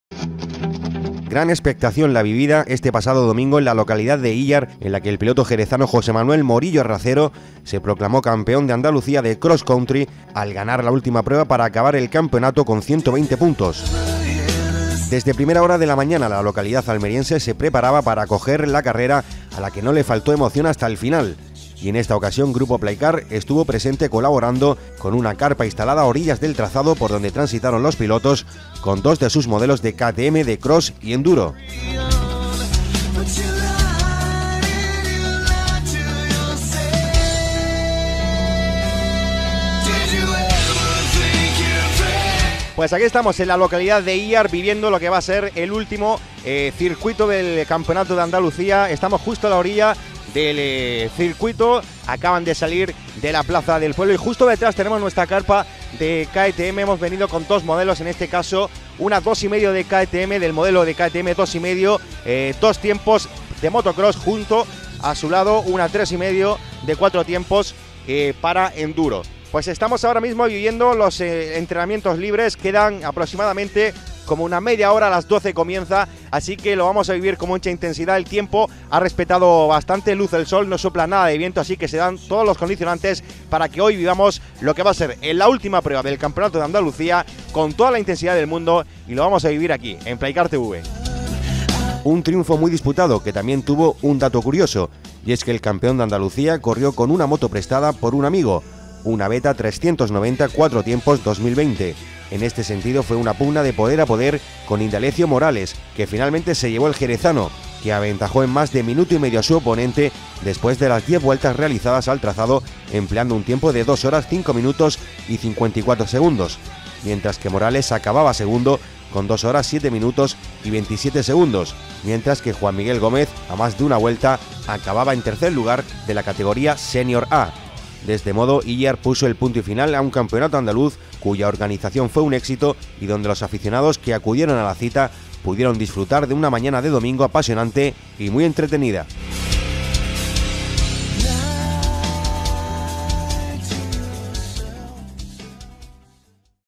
Cross Country Andaluz en Íllar
Gran expectación la vivida este pasado domingo en la  localidad de Íllar, en la que el piloto jerezano José Manuel Morillo Racero se proclamó campeón de Andalucía de Cross Country (XC) al ganar la última prueba para acabar el campeonato con 120 puntos.

Automóviles Playcar, Concesionario Multimarca